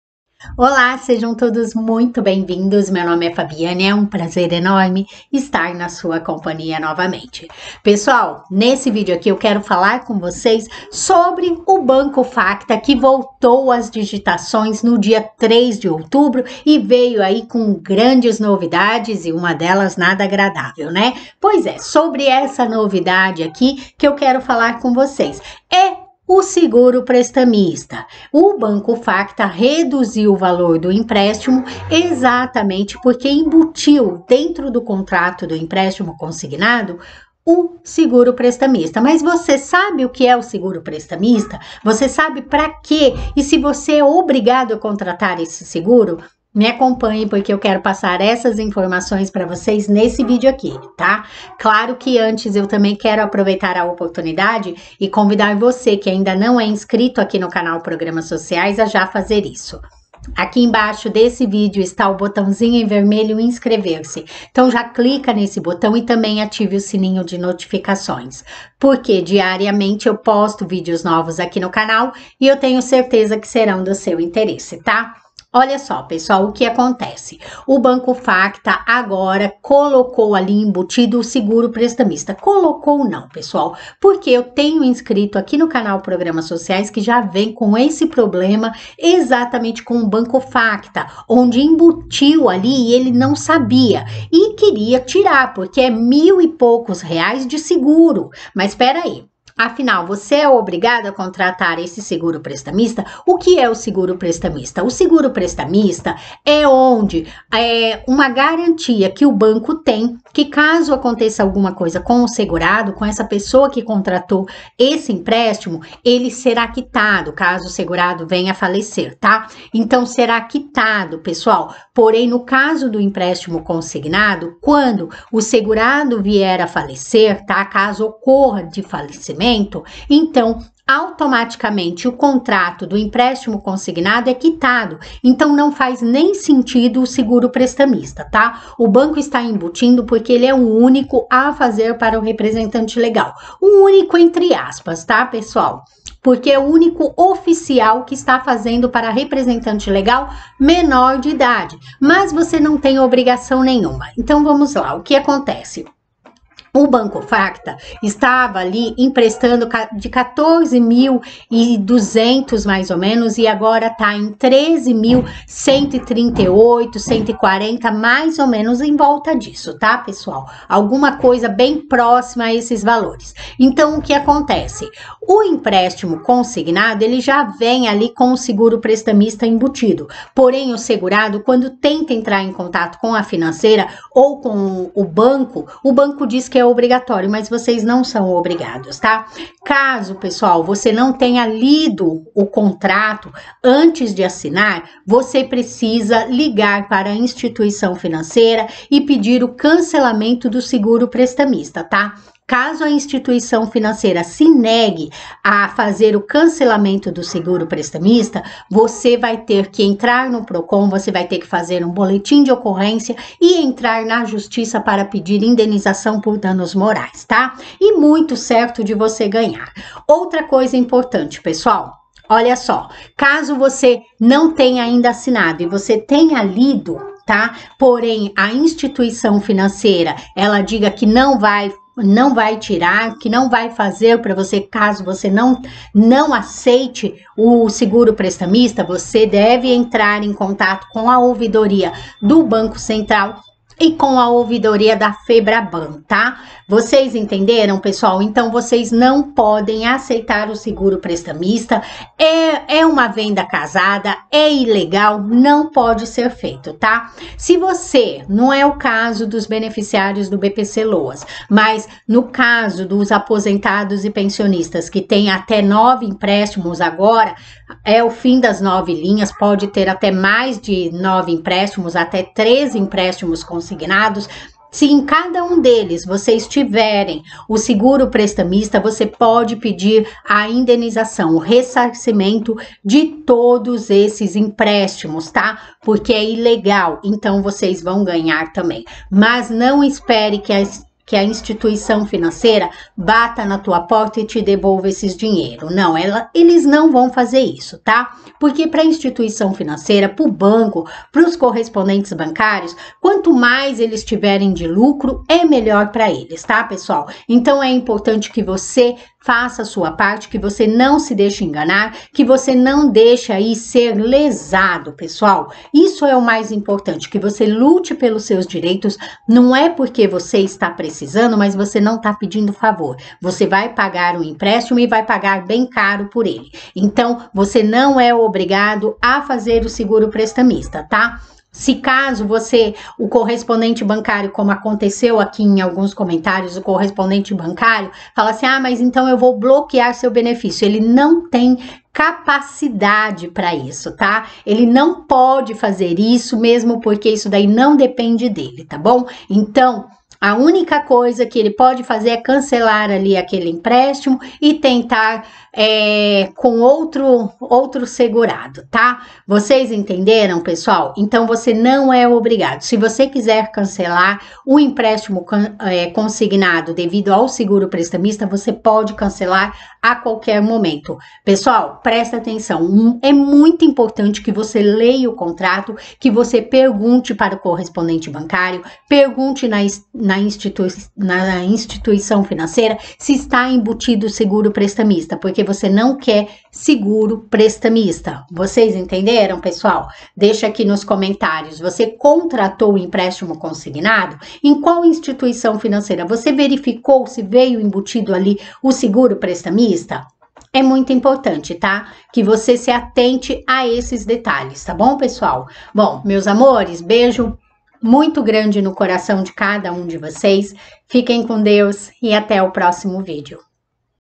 BANCO FACTA ESTÁ COBRANDO SEGURO PRSTAMISTA, O QUE FAZER!
REPRESENTANTE LEGAL É OBRIGADO A CONTRATAR O SEGURO PRESTAMISTA?